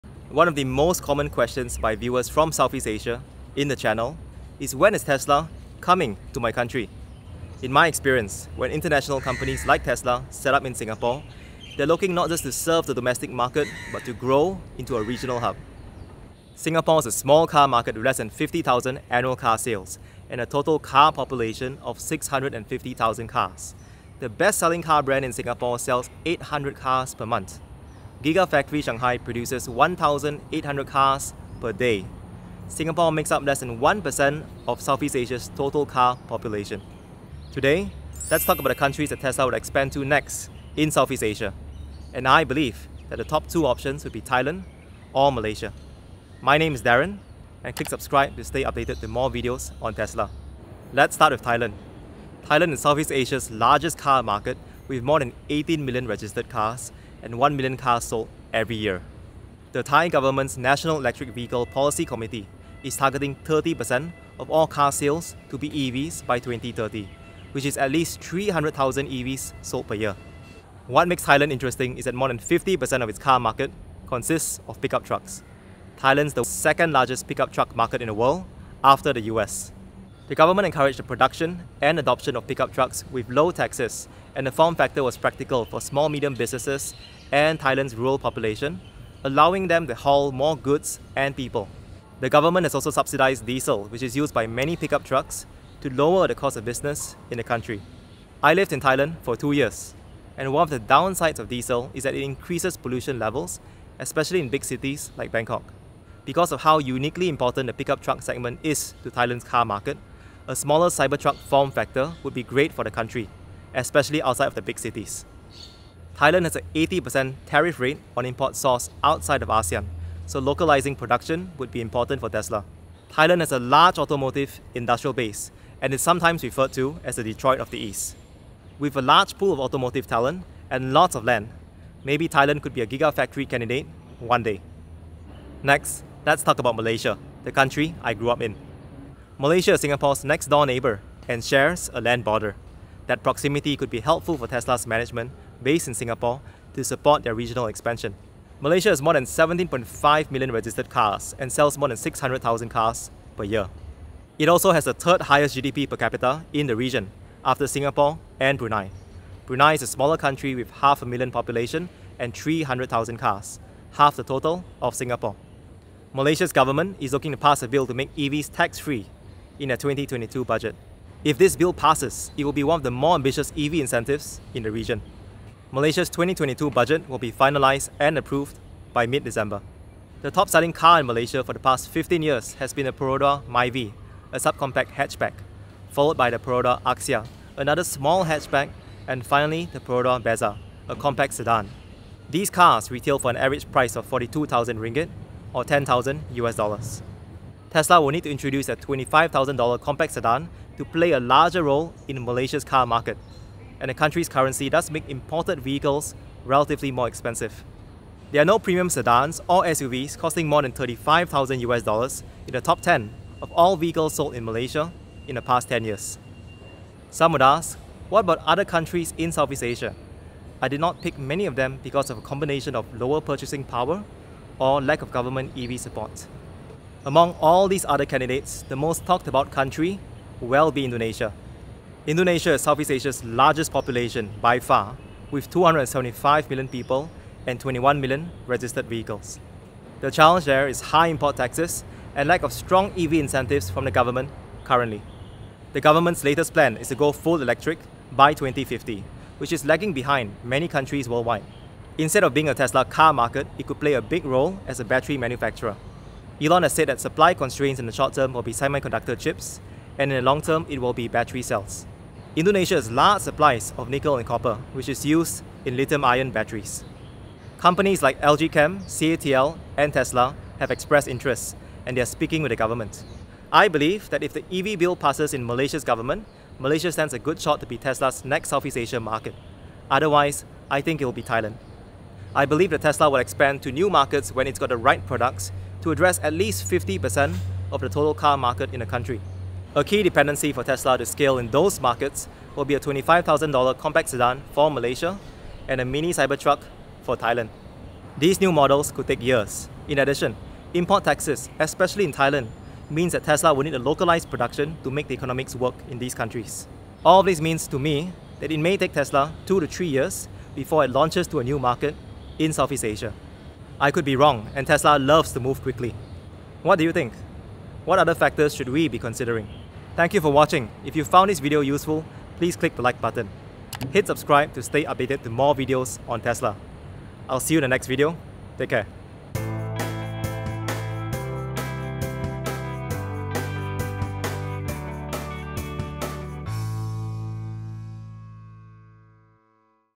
Tesla's Next Market in South East Asia?
Many viewers from South East Asia have asked when would Tesla enter their market. In this video, I share my hypothesis for the top 2 candidates for Tesla's future expansion in South East Asia - Thailand & Malaysia.

Reasons for Thailand:
- Largest car market in South East Asia (1m cars sold per year)
- Large automotive industrial base and talent pool

Reasons for Malaysia:
- EV tax-free bill in Budget 2022 can be a big catalyst for EVs
- Proximity to Tesla's Singapore Regional Headquarters

Timestamps:
0:00 Tesla in South East Asia
0:50 Reasons for Expanding to Thailand
2:15 Reasons for Expanding to Malaysia
3:36 Other Markets in South East Asia
4:33 Tesla's Future Expansion in South East Asia